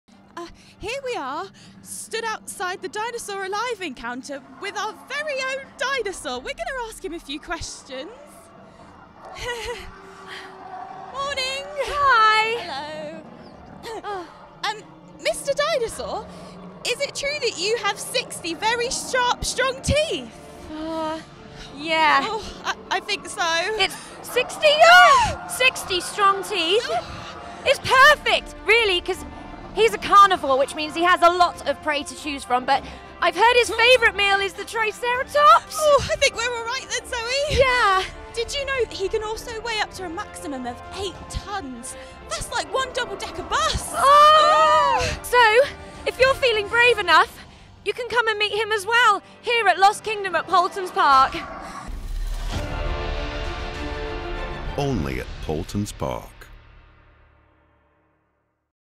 Alive! Dinosaur Encounter in Lost Kingdom at Paultons Park
An incredible opportunity to meet our tame(ish) walking realistic dinosaur at ALIVE - Dinosaur Encounter!